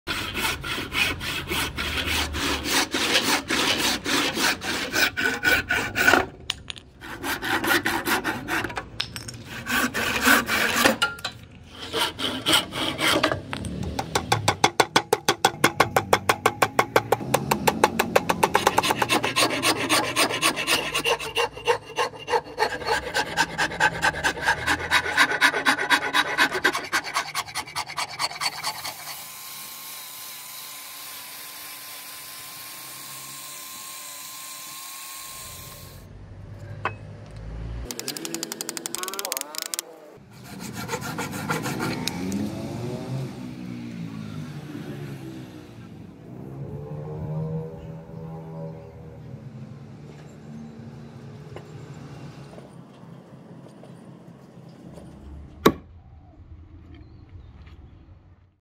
Making Super Cool Kitchen Axe For Grandma
Make awesome kitchen axes for Grandma. The handle is made of rosewood, designed to be unique but still usable. At the end of the video you will see this kitchen axe used by my grandmother for Thai style grilled pork steak, it was delicious and we all enjoyed it. thank you everyone for watching and see you next project . Have a nice day.